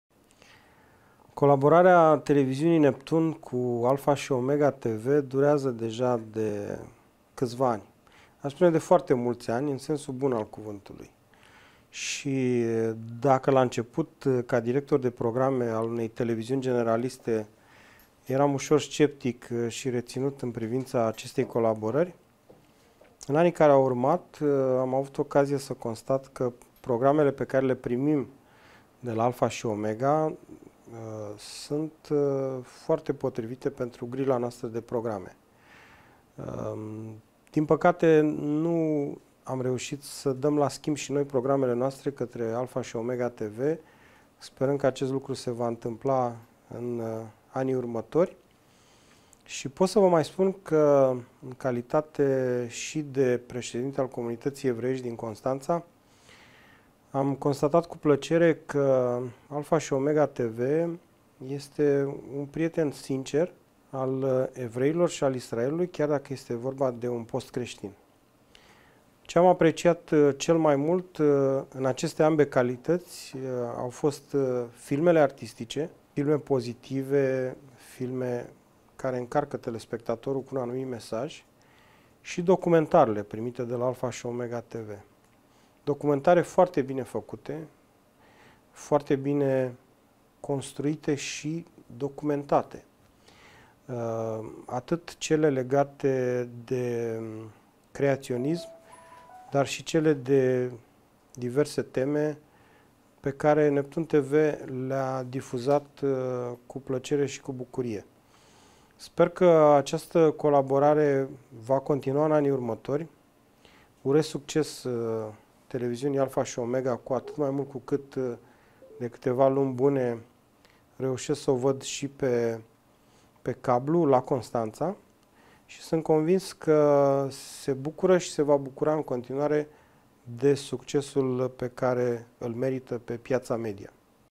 Sorin Ionescu (Neptun TV) - Despre colaborarea cu Alfa Omega TV si programele crestine pe Neptun TV
Dacă la început, ca director de programe al unei televiziuni generaliste, eram sceptic în privința acestei colaborări, în anii care au urmat, am constatat că programele de la Alfa Omega TV sunt foarte potrivite pentru grila noastră de programe.